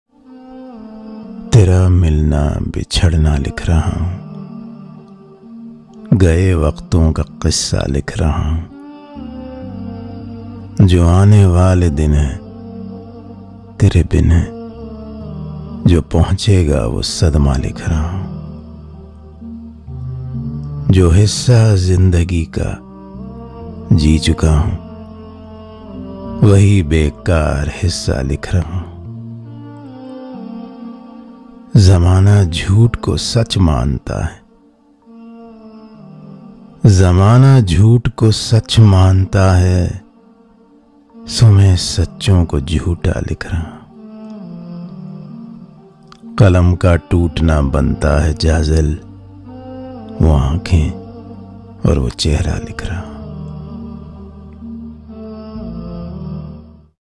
Tera Milna Bicharna Likh Raha Hoon |Sad Urdu Poetry | Beautiful Urdu Poetry
Poet: Jazil
Voice: Anwer Olia

Music used is from YouTube Library

All the content in this video is the sole property of Adabi Log